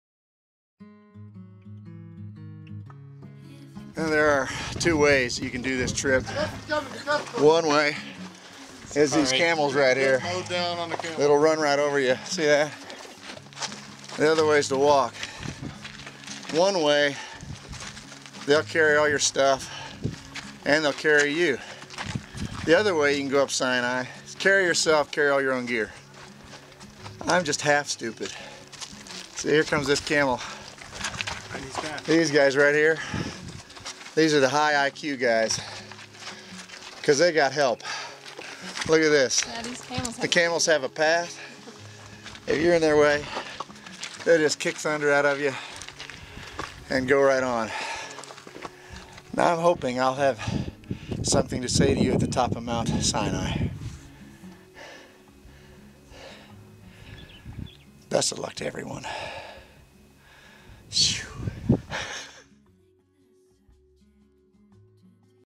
Mount Sinai
Often times we need reminded that we need help - that we need each other. During Mark's trip to Egypt, this became clear to him as he was climbing Mount Sinai.